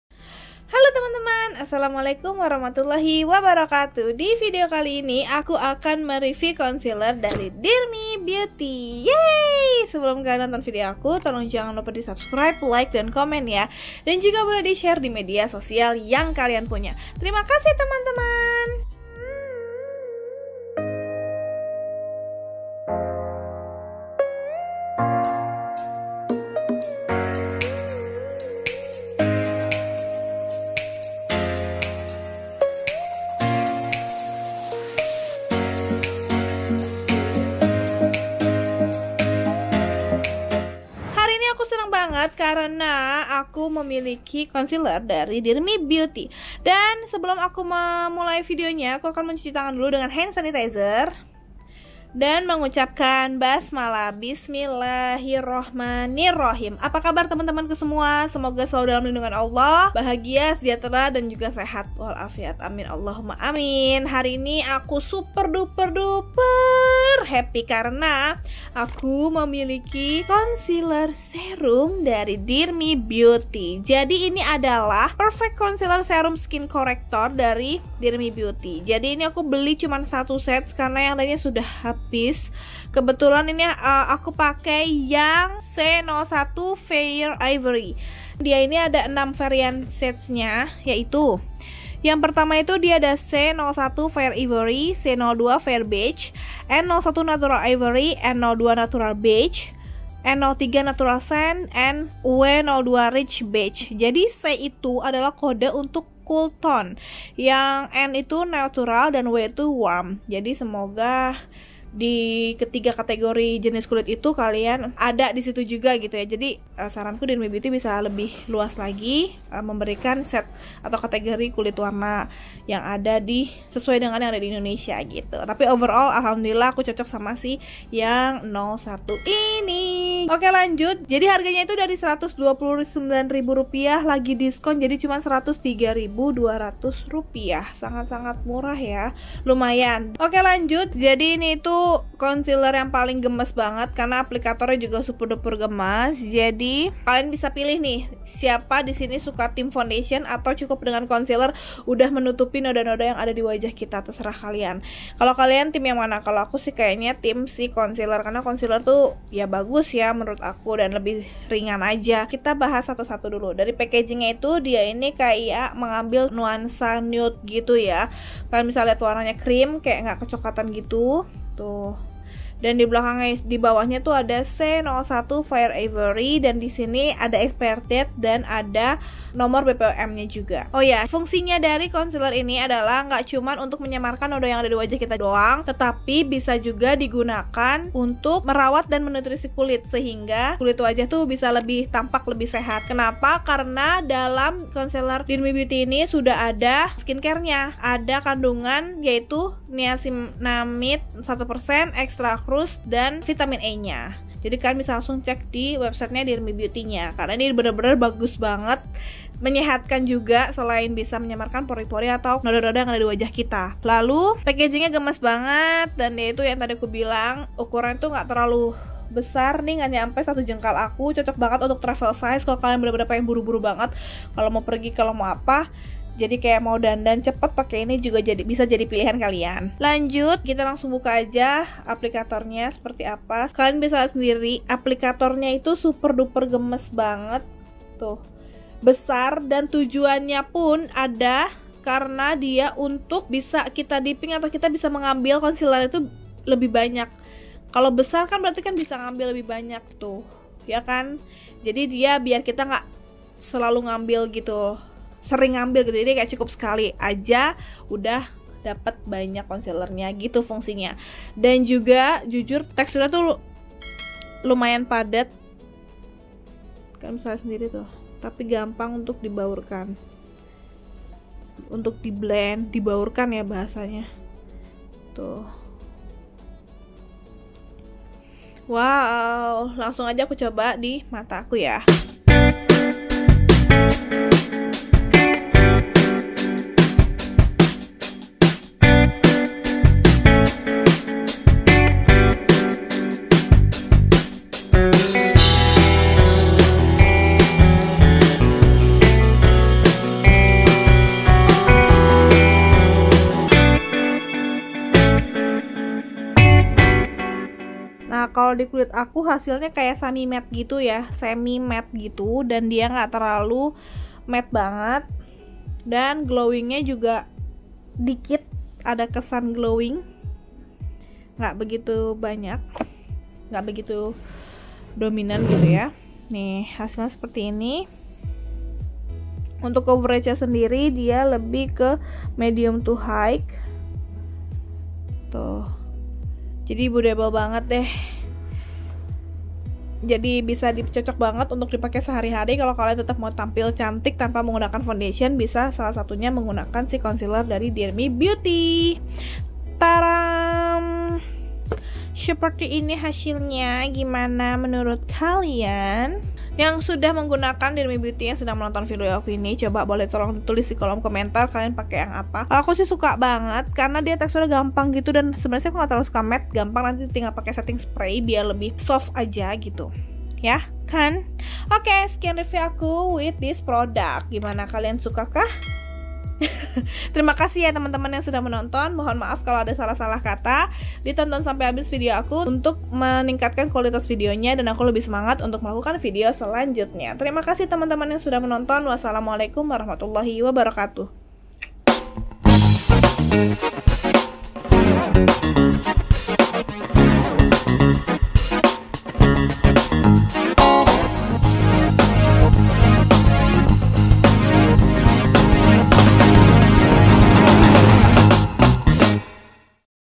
CONCEALER SERUM DEAR ME BEAUTY | deirarazzaaqdarsono
Yeay ! kali ini aku akan review lagi, kali ini adalah concealer dearme beauty, kalo kalian tim yang mana ?

Chapters
00:00 Intro
01:00 About product
02:10 Product Price
02:42 Packaging Product
04:41 Test Product
06:11 Outro

ABOUT MY CHANNEL:
Makeup, anywhere and everywhere

concealer best how to apply concealer concealer review ilia beauty ilia true skin serum concealer